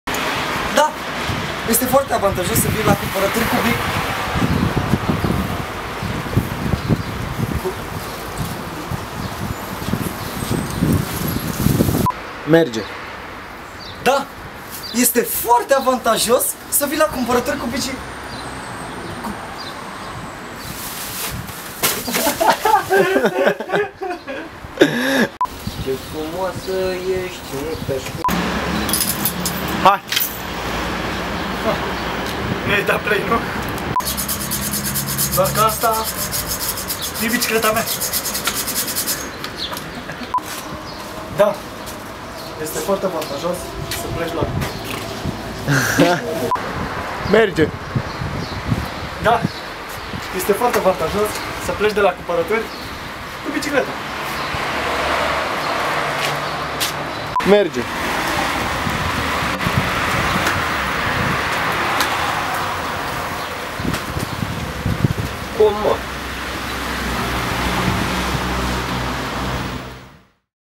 Mega Courage - bloopers
scene din supermegaultramagicaextraordinarafantasticamirobolanta productie MEGA COURAGE concurenta la Grand Spoof 2012, care parodiaza reclama de la Mega Image. Enjoy and dont forget to vote.